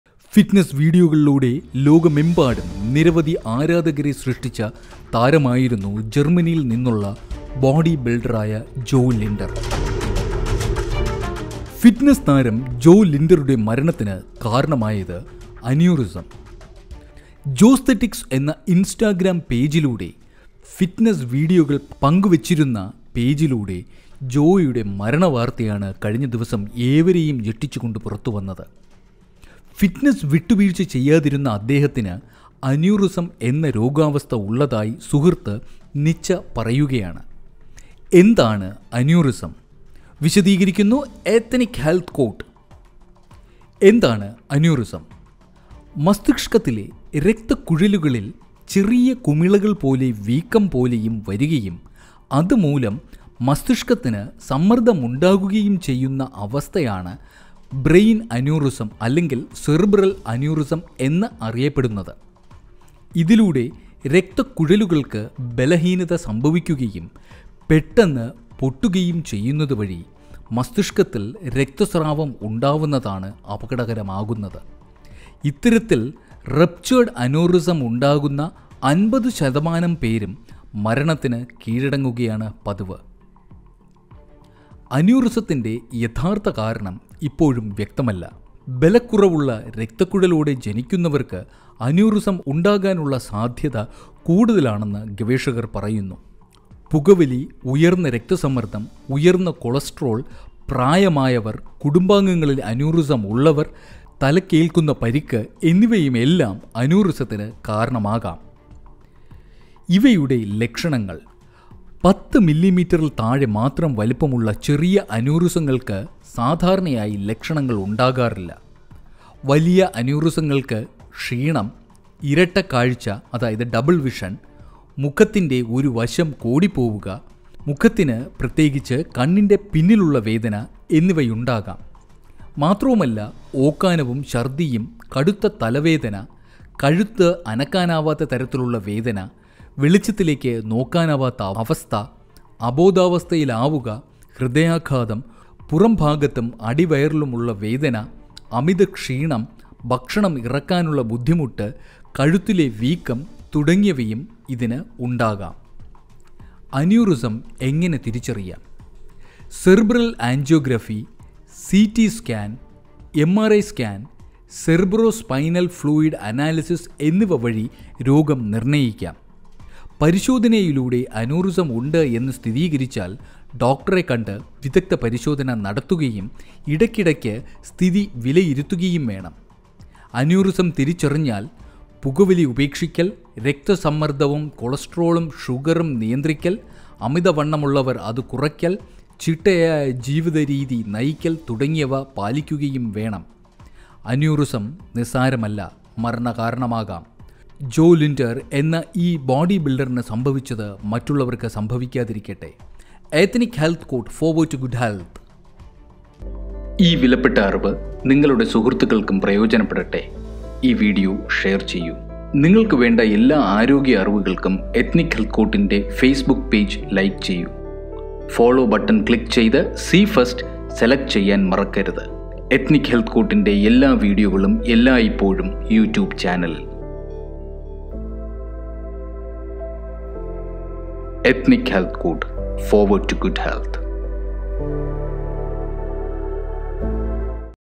30-ാം വയസ്സിൽ ബോഡി ബിൽഡറുടെ മരണം | കാരണം അന്യൂറിസം | Ethnic Health Court
ഫിറ്റ്നസ് വീഡിയോകളിലൂടെ ലോകമെമ്പാടും നിരവധി ആരാധകരെ സൃഷ്ടിച്ച താരമായിരുന്നു ജർമനിയിൽ നിന്നുള്ള ബോഡിബിൽഡറായ ജോ ലിൻ‍ഡ്നർ. ജോസ്തെറ്റിക്സ്(Joesthetics) എന്ന ഇൻസ്റ്റഗ്രാം പേജിലൂടെ ഫിറ്റ്നസ് വീ‍ഡിയോകൾ പങ്കുവെച്ചിരുന്ന ജോയുടെ   മരണവാർത്തയാണ് കഴിഞ്ഞ ദിവസം ഏവരെയും ഞെടിച്ച് കൊണ്ട് പുറത്തു വന്നത്. ഫിറ്റ്നസിൽ വിട്ടുവീഴ്ച്ച ചെയ്യാതിരുന്ന അദ്ദേഹത്തിന്  അന്യൂറിസം എന്ന രോഗവസ്ഥ ഉള്ളതായി  സുഹൃത്തായ നിച്ച പറയുകയാണ്.  എന്താണ് അന്യൂറിസം.? വിശദീകരിക്കാം എത്‌നിക് ഹെൽത്ത്‌ കോർട്ട്.